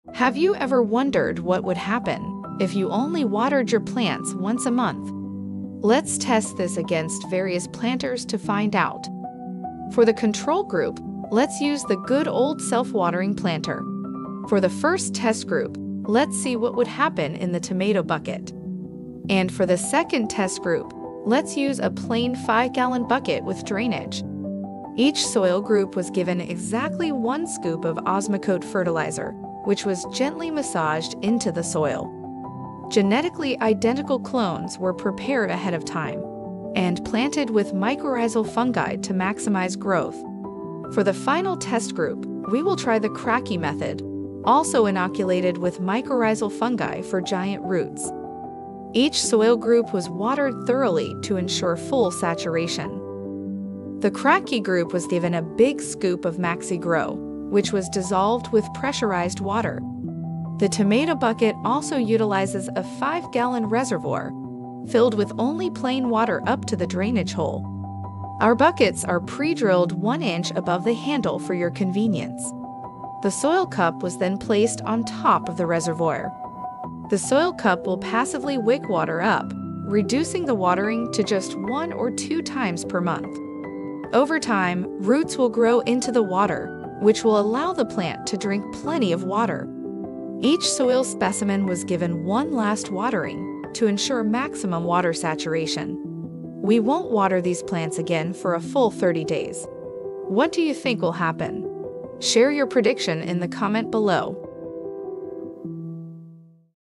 What If You Water Your Plant Once a Month? Part 1 | Soil Cup Self Watering Bucket Insert
The Soil Cup Self Watering Bucket Insert allows you to grow weed, herbs, & vegetables faster with only one watering per month, making gardening super easy!

What If You Only Watered Your Plant Once a Month? (Setup)
Have you ever wondered what would happen,
If you only watered your plants once a month?
Let’s test this against various planters to find out.
For the Control Group, let’s use the good old self watering planter.
For the 1st Test Group, let’s see what would happen in the Tomato Bucket.
And for the 2nd Test Group, let’s use a plain 5-gallon bucket with drainage.
Each soil group was given exactly 1 scoop of Osmocote fertilizer,
Which was gently massaged into the soil.
Genetically identical clones were prepared ahead of time,
And planted with mycorrhizal fungi to maximize growth.
For the final Test Group, we will try the Kratky Method,
Also inoculated with mycorrhizal fungi for giant roots.
Each soil group was watered thoroughly to ensure full saturation.
The Kratky group was given a big scoop of MaxiGro,
Which was dissolved with pressurized water.
The Tomato Bucket also utilizes a 5-gallon reservoir,
Filled with only plain water up to the drainage hole.
Our buckets are pre-drilled 1” above the handle for your convenience.
The Soil Cup was then placed on top of the reservoir.
The Soil Cup will passively wick water up,
Reducing the watering to just 1 or 2 times per month.
Over time, roots will grow into the water,
Which will allow the plant to drink plenty of water.
Each soil specimen was given one last watering,
To ensure maximum water saturation.
We won't water these plants again for a full 30 days.
What do you think will happen?